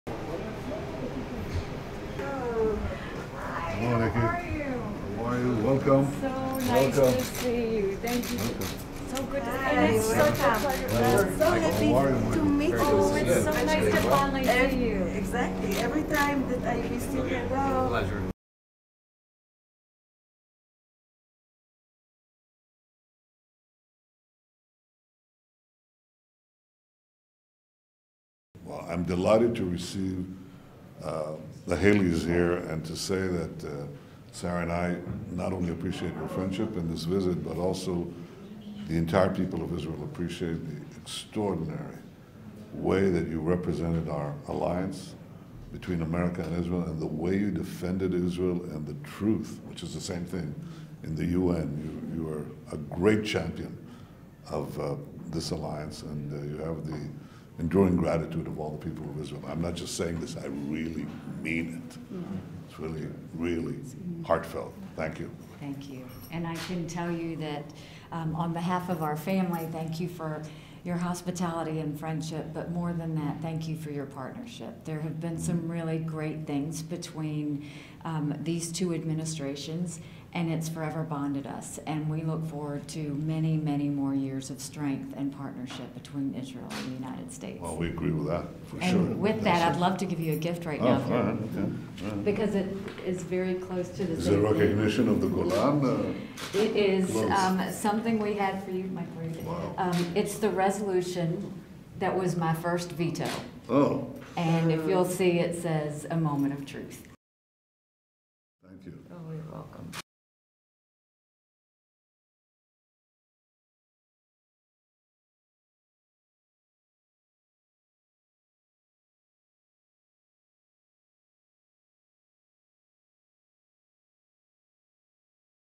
PM Benjamin Netanyahu Hosts Former US Ambassador to the UN Nikki Haley
ראש הממשלה בנימין נתניהו ורעייתו הגב' שרה נתניהו אירחו לארוחת צהריים את שגרירת ארה"ב באו"ם לשעבר ניקי היילי ובעלה מייקל היילי, במעון רה"מ בירושלים.

Prime Minister Benjamin Netanyahu and his wife Sara hosted former US Ambassador to the UN Nikki Haley and her husband Michael Haley for lunch at the Prime Minister's residence in Jerusalem.